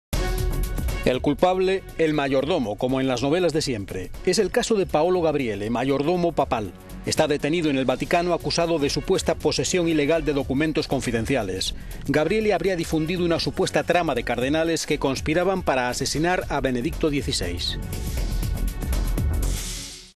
El mayordomo del Papa detenido por revelación de secretos
El culpable, el mayordomo, como en las novelas de siempre. Es el caso de Paolo Gabriele, mayordomo papal. Está detenido en el Vaticano acusado de supuesta posesión ilegal de documentos confidenciales. Gabriele habría difundido una supuesta trama de cardenales que conspiraban para asesinar a Benedicto XVI. Puede ser condenado hasta a 30 años de cárcel vaticana por violación de correspondencia del jefe del Estado.